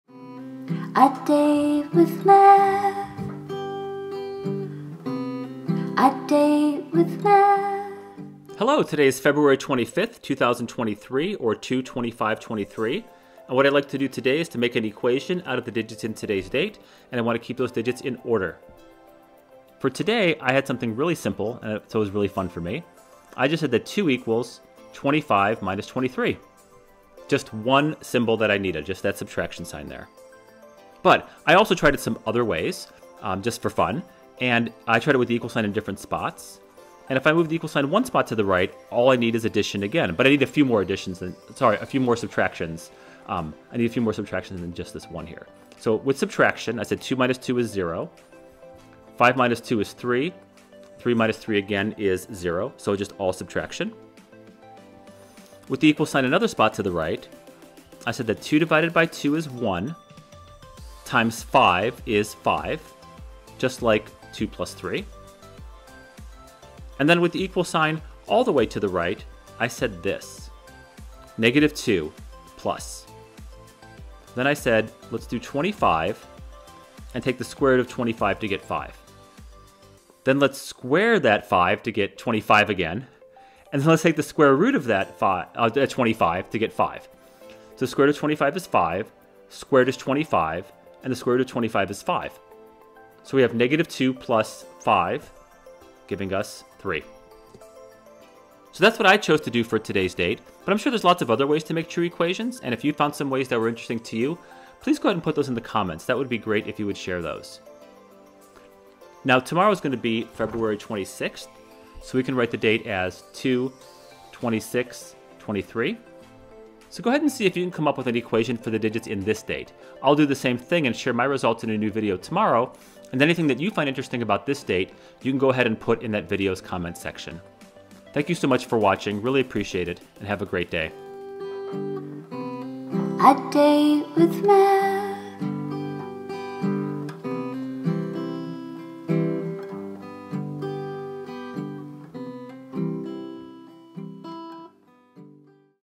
A Date with Math: 2/25/23
The Math Institute presents A Date with Math! Students and teachers can delight in puzzles dealing with today's date. Students can practice order of operations and use different math symbols to make equations. Leave your solution in the comments below!

A factorial has the symbol ! and means to multiply a number by every whole number down to 1. So 3!= 3x2x1=6, 4!=4x3x2x1=24, and 5!=5x4x3x2x1=120. So you can see the numbers can get bug quickly. Factorial is used in many areas of mathematics, especially in probability where we often want to multiply numbers that fit this pattern.